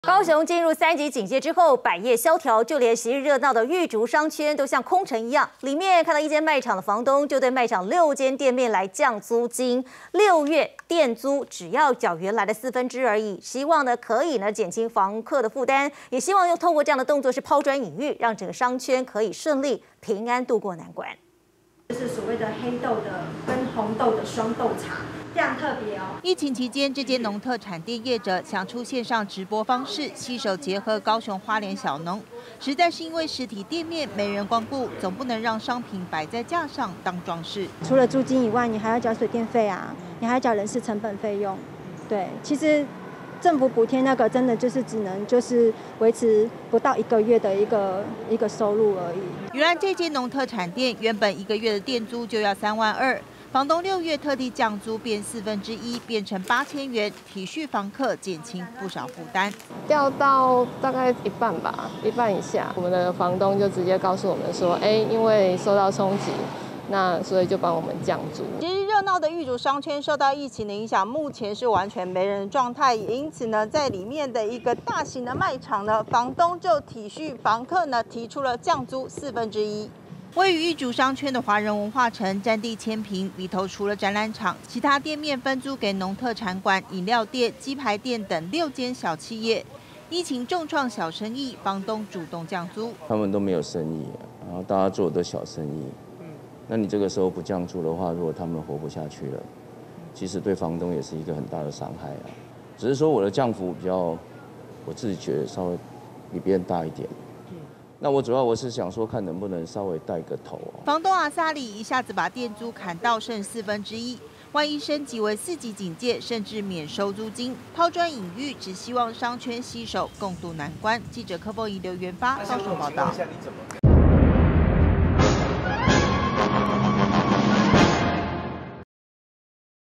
揪甘心！ 「玉竹商圈」業績慘淡 房東降租金剩1／4
高雄進入三級警戒後，百業蕭條，就連昔日熱鬧的玉竹商圈，都猶如空城，裡頭一間賣場房東，就對賣場六間店面降租，6月店租只要繳原來的四分之一，減輕房客負擔，也希望拋磚引玉，讓商圈度過難關。